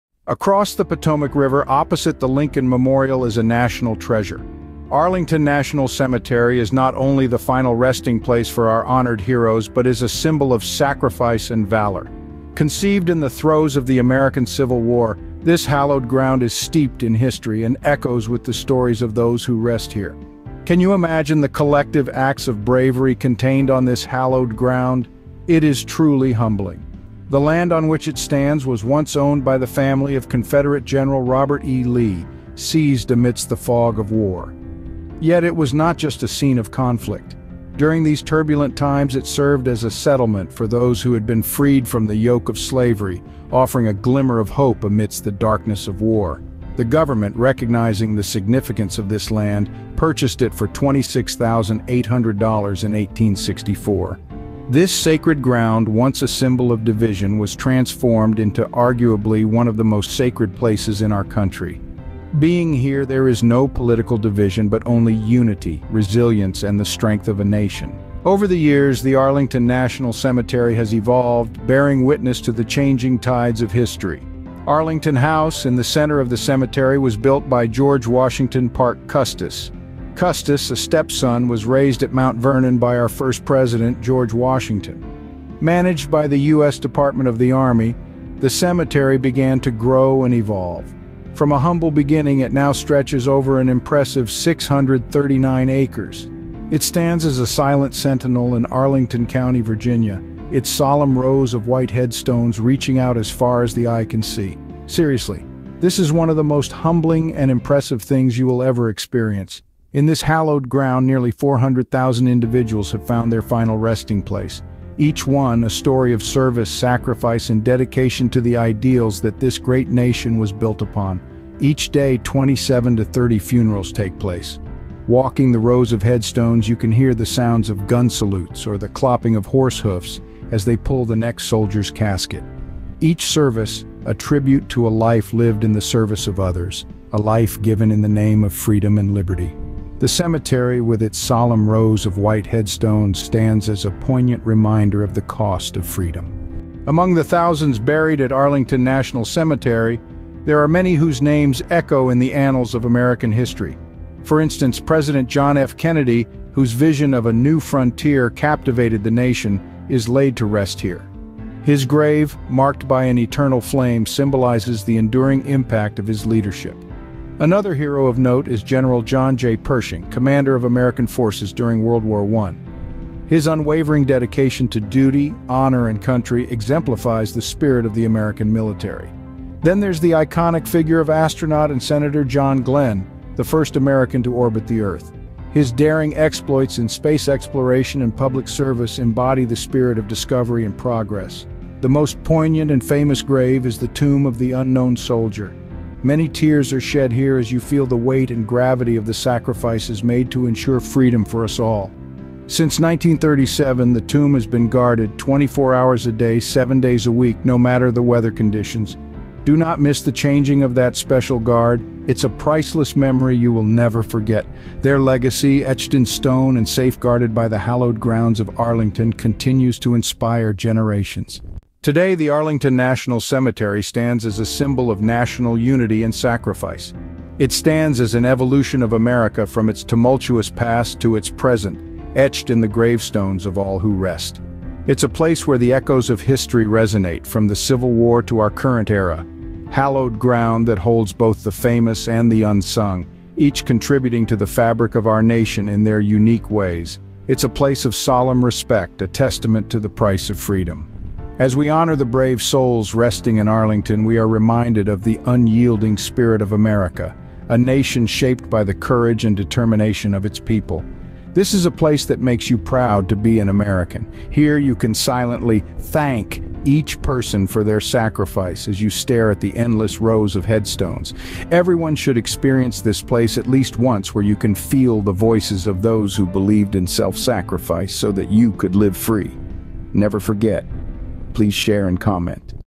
Discover the Sacred Grounds of Arlington National Cemetery
Join us as we explore the sacred grounds of Arlington National Cemetery, a place of national pride and rich history. Walk among the gravestones of America's brave soldiers and discover the powerful stories behind them. This is a must-visit destination for anyone wanting to truly understand the sacrifices of our nation's heroes.
Learn the history of Arlington National Cemetery.  A place that makes you proud to be an American.